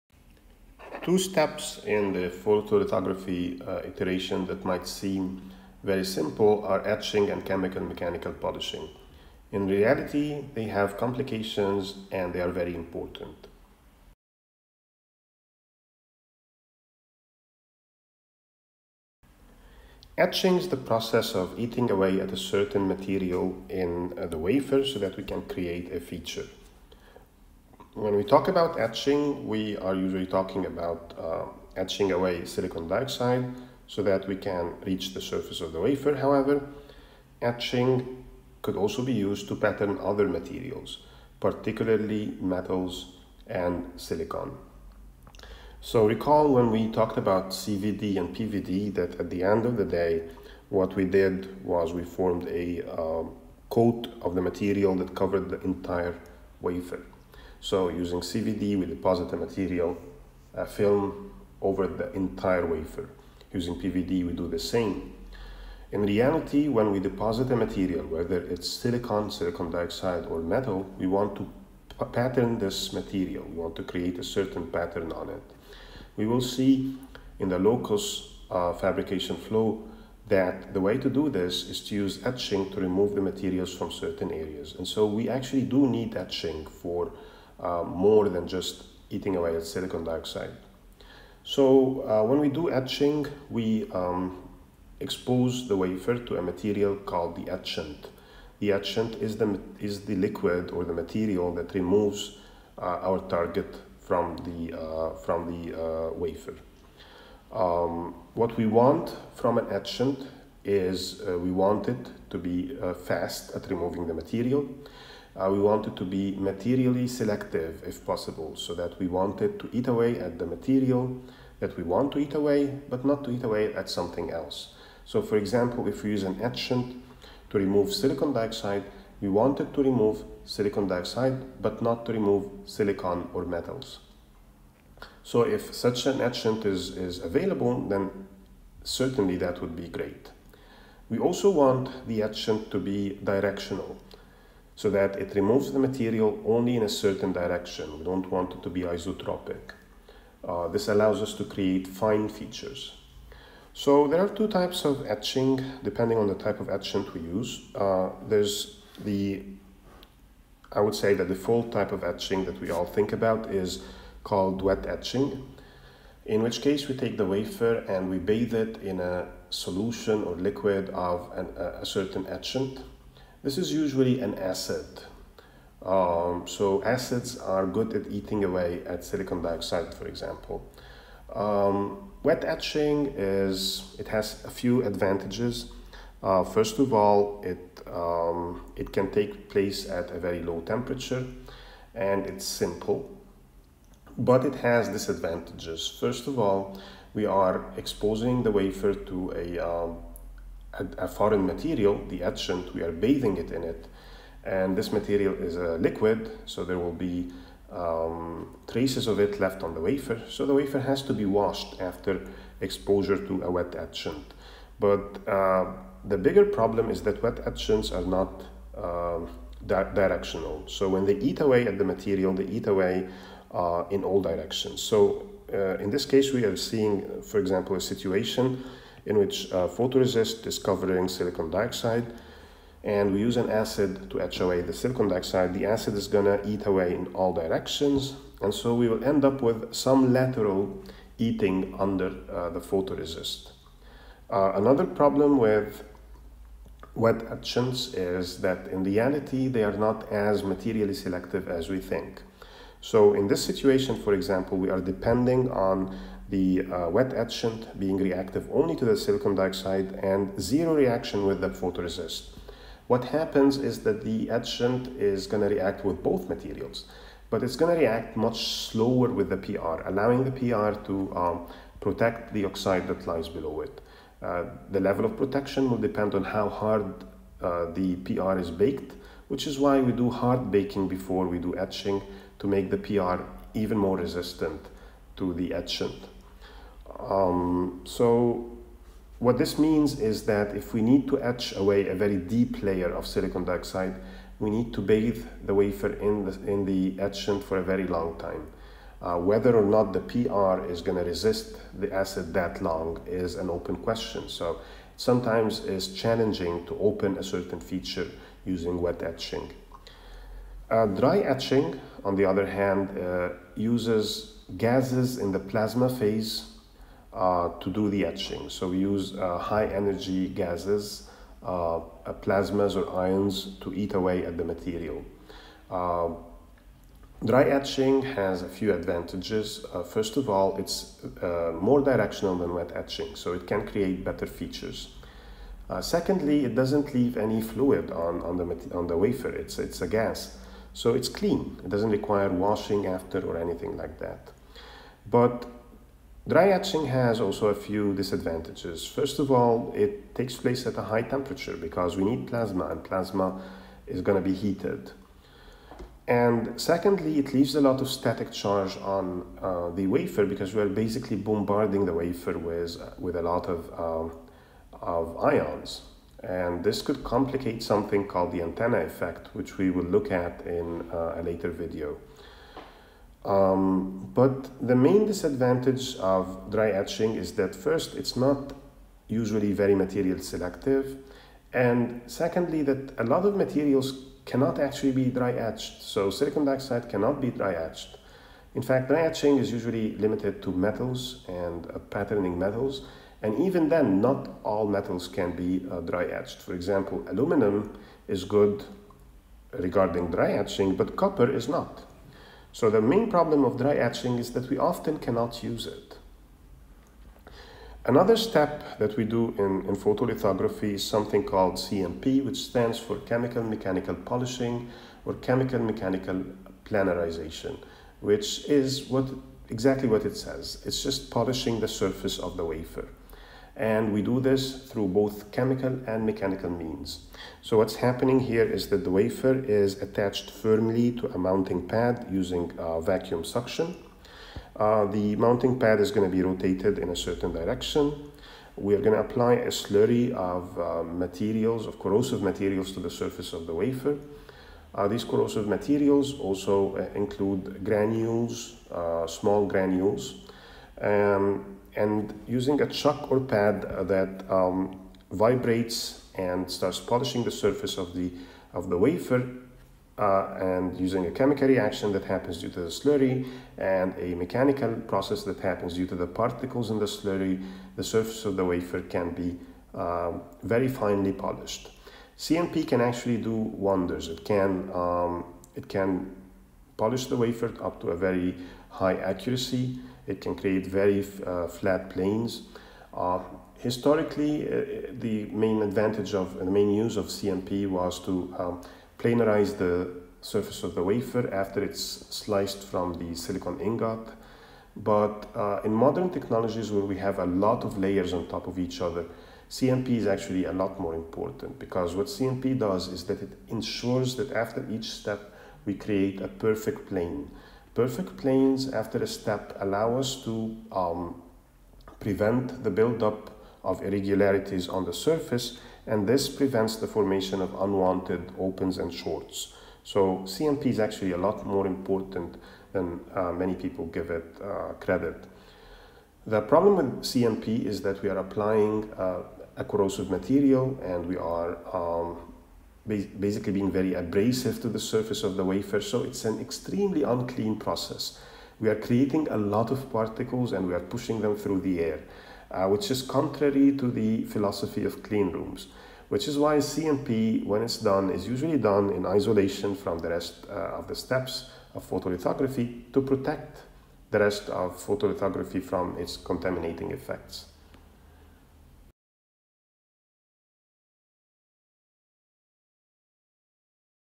7.8. Etching & CMP
Etching and CMP are fairly simple. The first eats away at exposed materials, the second polishes an irregular surface. However, both operations are extremely important and if not handled properly can end up ruining the entire flow.